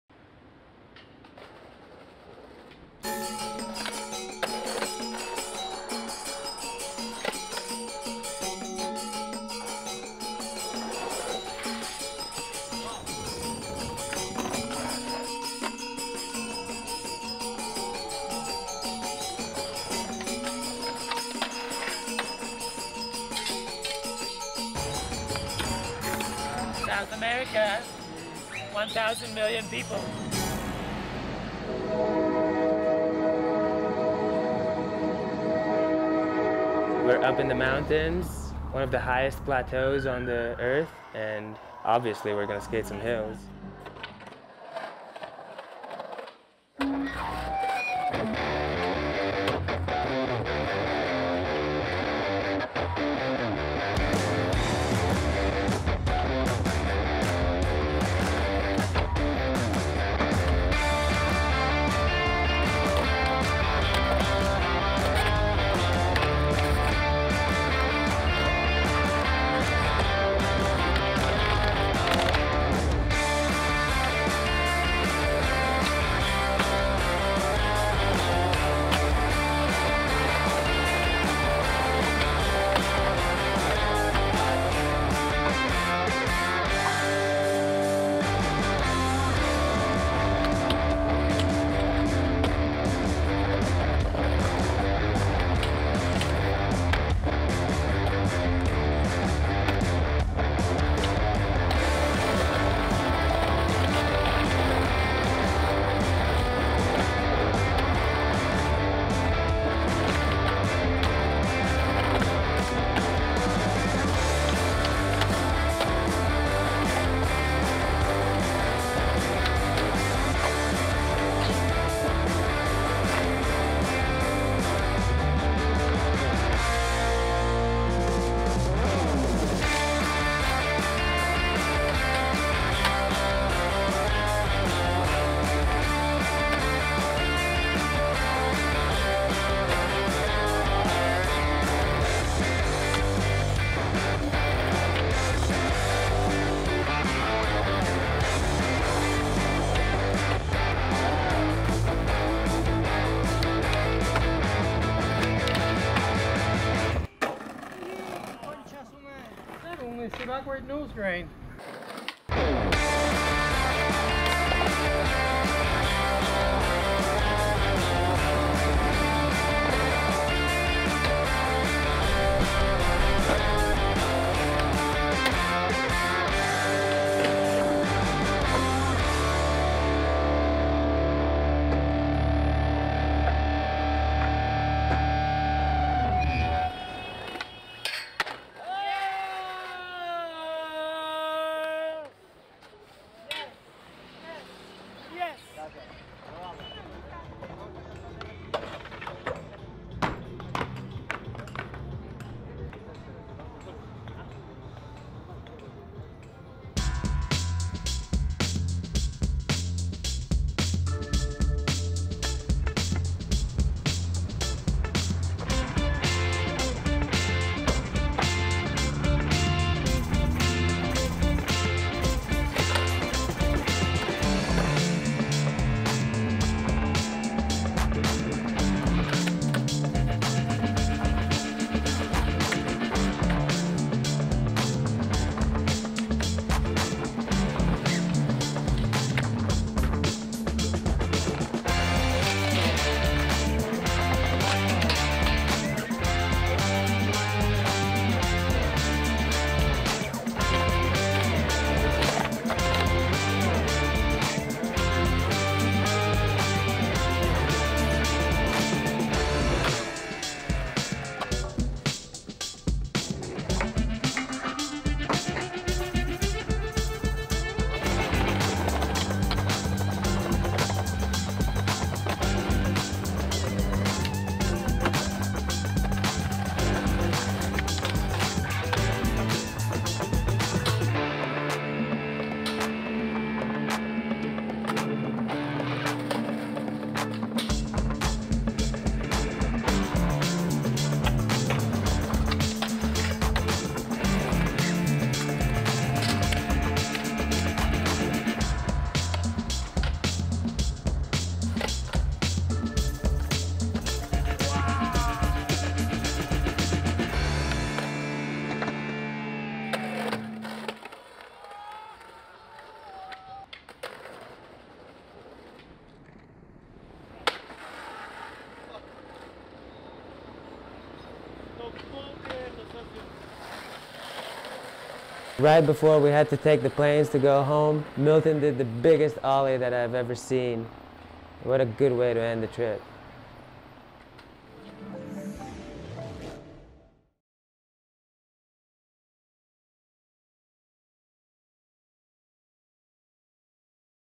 An Unrepeatable Skate Mission To Bolivia & Peru | THE INCA TRAIL
Enjoy a condensed hammer remix of Milton Martinez, Madars Apse, Thaynan Costa and Angelo Caro's unrepeatable skate mission through Bolivia and Peru.

Skating hard on a trip to a new destination requires a particular mindset. You have to be able to deal with what’s in front of you there and then. Spot repairs have to be of the jerry-rigged, ad-hoc variety, and the chance to come back tomorrow is usually off the table. Tomorrow you'll be somewhere down the road, nursing a heel bruise and wondering where dinner is going to come from.

It is not for everyone, to be sure. Travelling and skating is a real test of resilience. It becomes a character study of who can roll outside of their comfort zone. You’ve got to choose wisely with whom you travel and where you go.

For the first instalment in our action remix series of classic Skate Escapes we bring you a hammer digest from the thin air of our high-altitude skate mission through Bolivia and Peru on the Inca Trail.